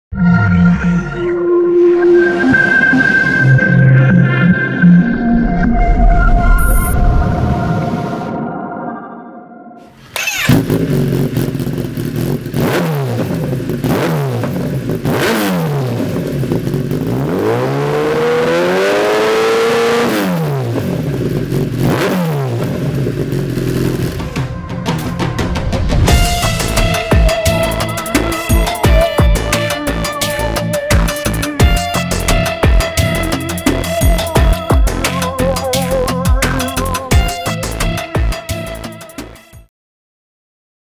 Lextek XP8C Exhaust Sound: Kawasaki ZX1000 2013
This video is to demonstrate the sound of a Lextek XP8C Exhaust.
The bike this was fitted to was a Kawasaki ZX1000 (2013).